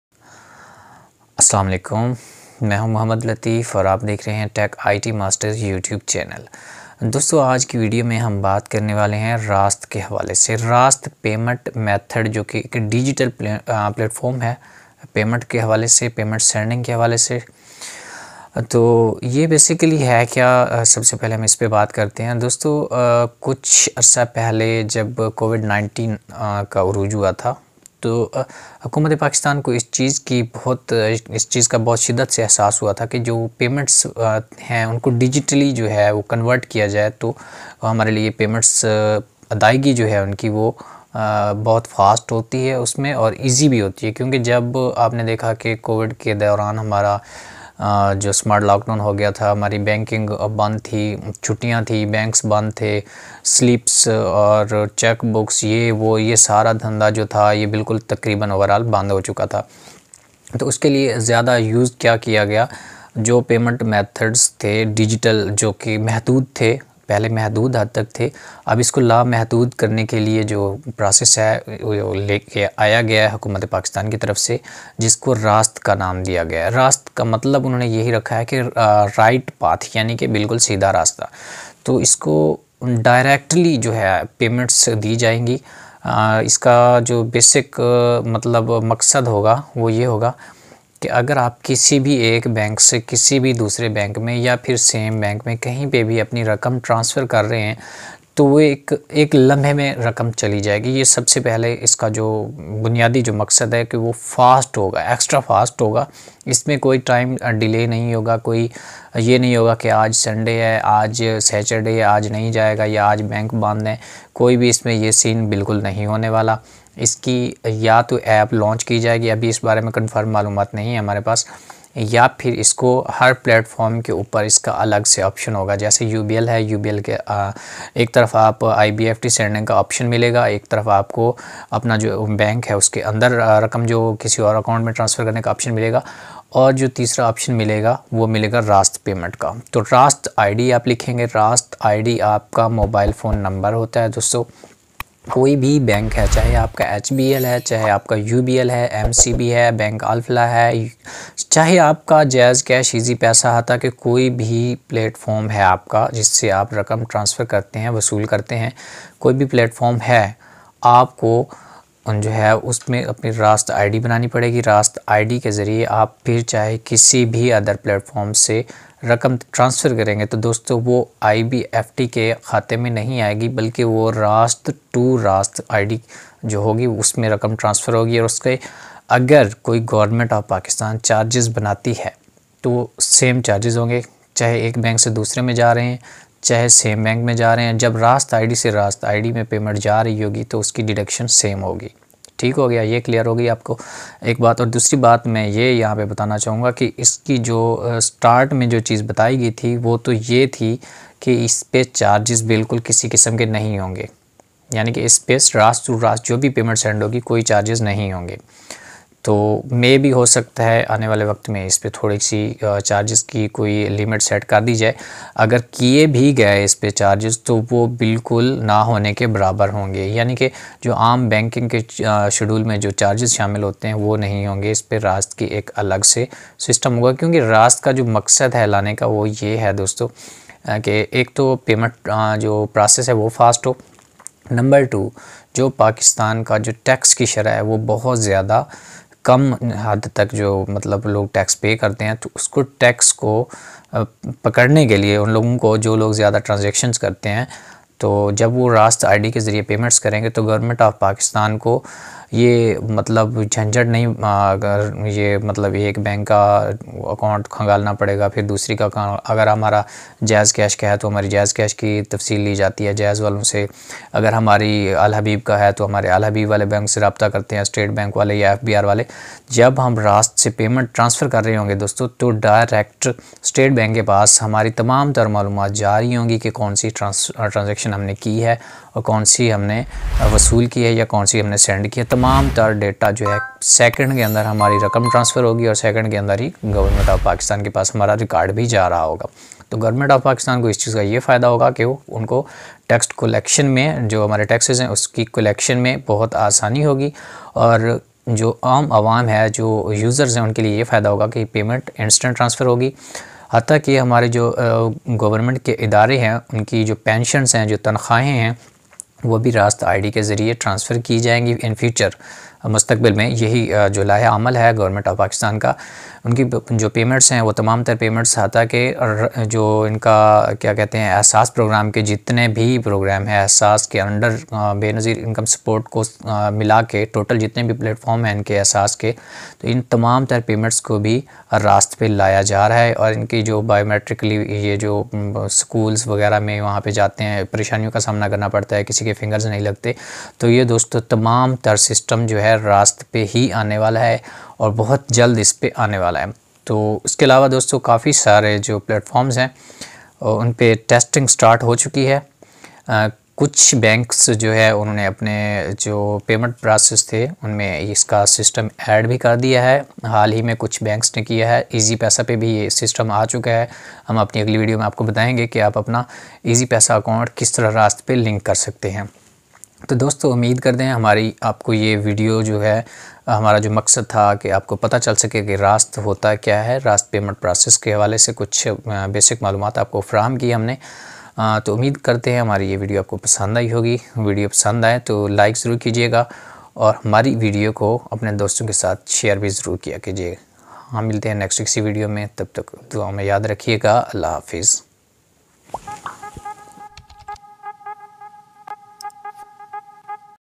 What is Raast Payment System | Benefits Of Raast | Pakistan's Digital Instant Payment System
Learn This Video is About What is Raast Payment System in Pakistan. Raast is the Pakistan's Digital Instant Payment System. Raast Instant Payment System Kya Hai aur Isko Kese Use Karty Hain. State Bank Of Pakistan Launches Pakistan's Digital Instant Payment System For All Banks User's.

                          《FOLLOW US》

                     《VIDEO CREDITS》

► Voice Over: Muhammad Latif Khakh

► Editing: Muhammad Shahzad Khakh

Like This Video 👍 & Leave Your Good Comment 📝 In The Comment Box. 💞